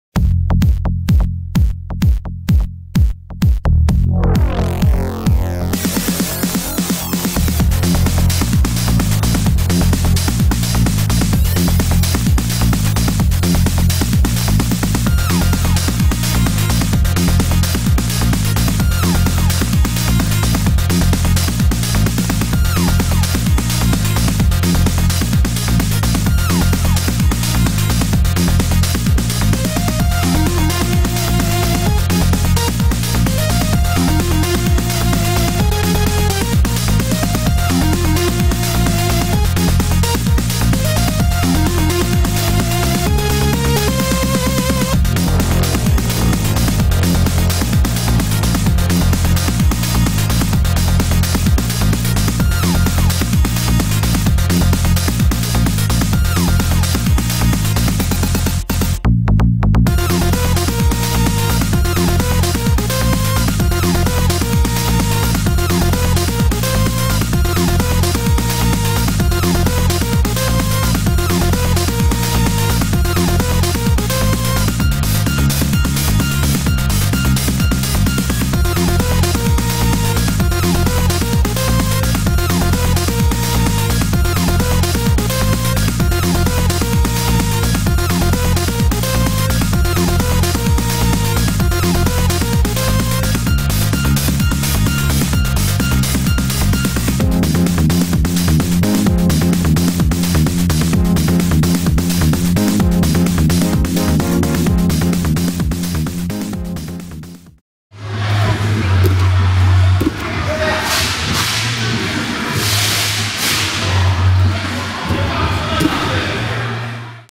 Power Gymnastics Reel
Some scrap footage from 2012. Not a comprehensive catalogue but a demo.